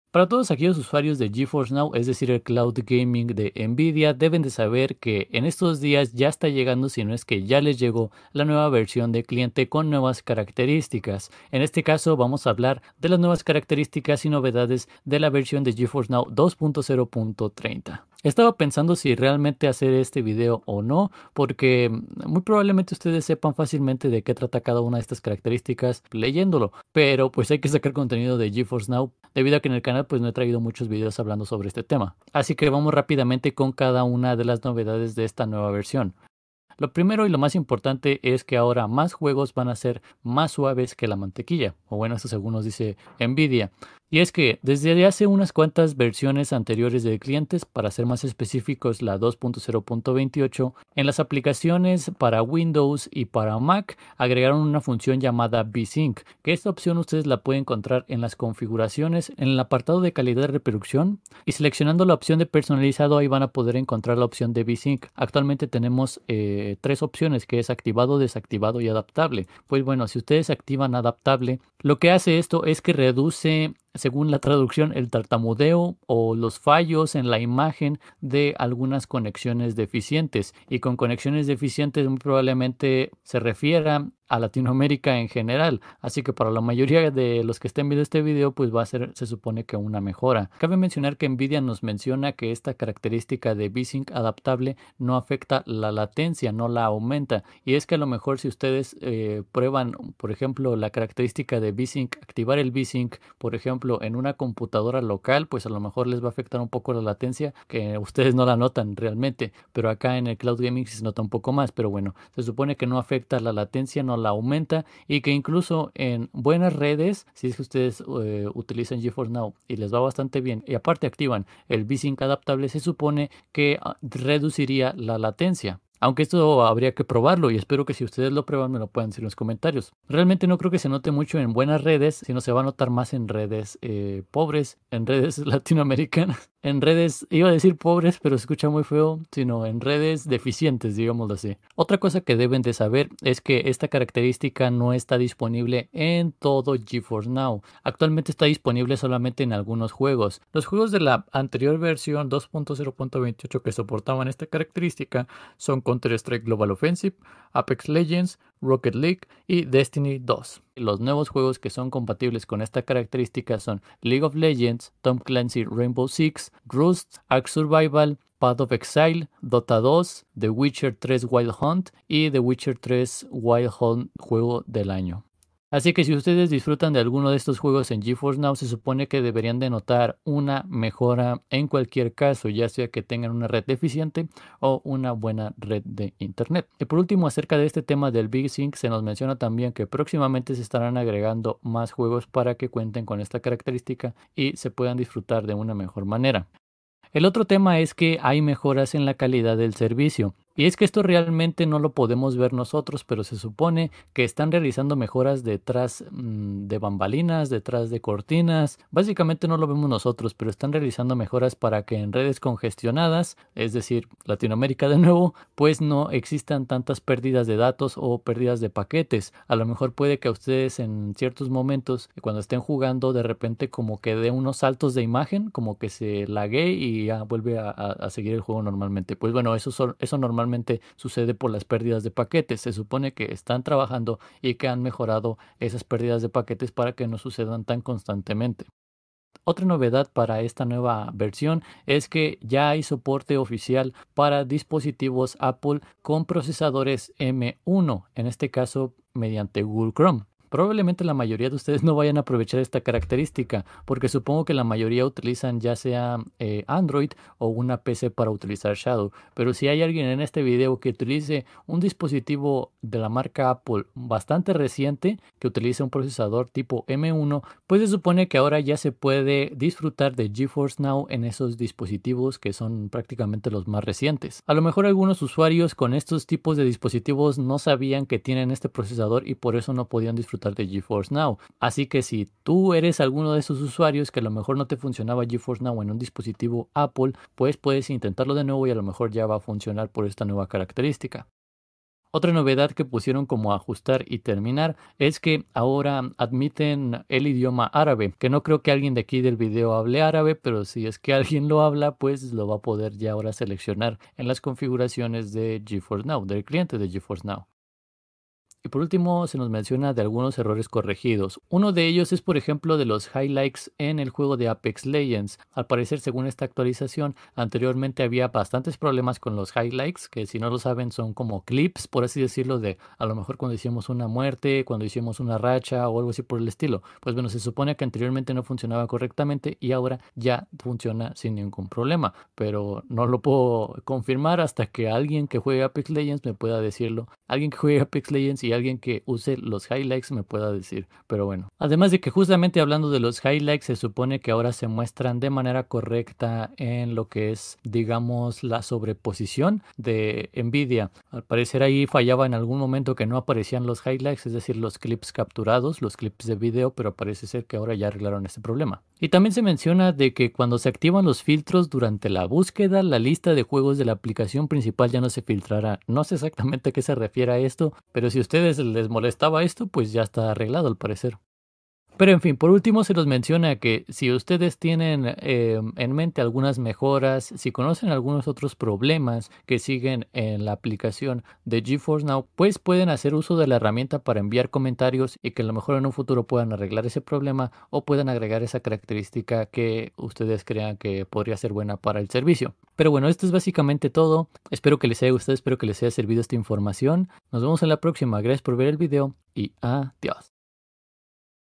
🎮 MENOS LATENCIA Y PACKET LOSS en Geforce Now (2.0.30)[2021]
Una nueva version del cliente de Geforce Now ya esta disponible y viene con nuevas novedades que les explicare en el video...